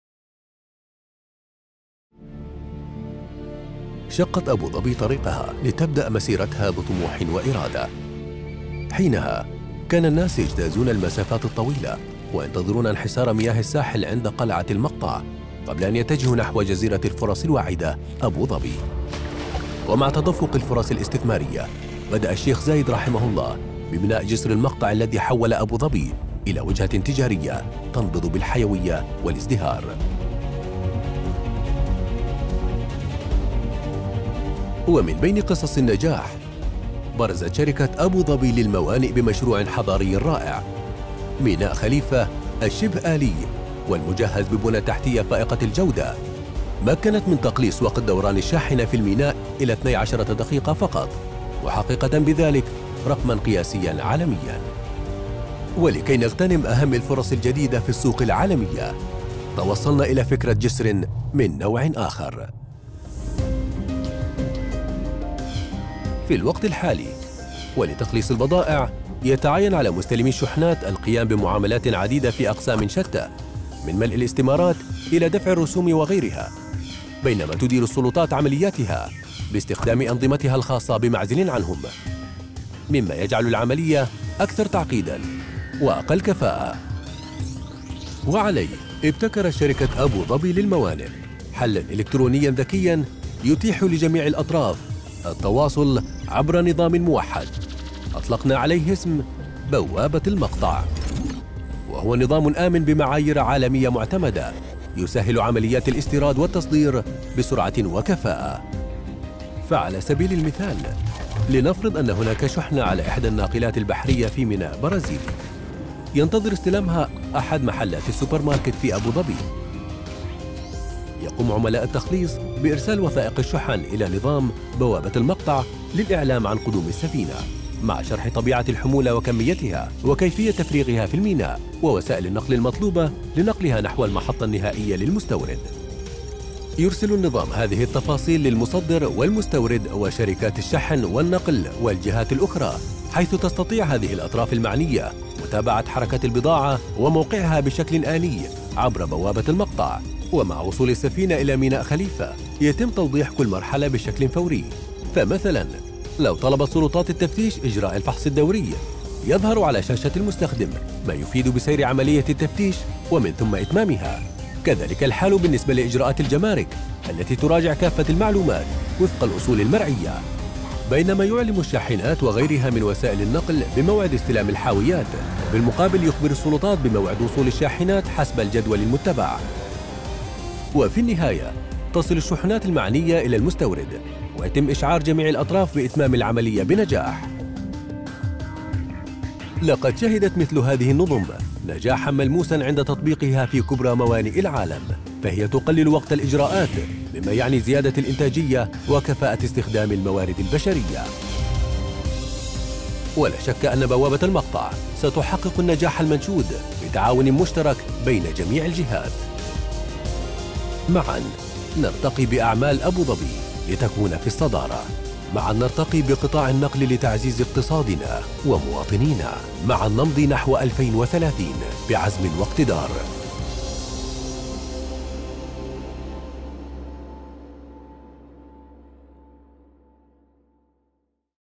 MAQTA Fully automated Port Community System (PCS) by ADPC
Introducing the most technologically advanced community system in Abu Dhabi Ports. MAQTA Gateway is fully integrated automated system that eases Import and Export procedures.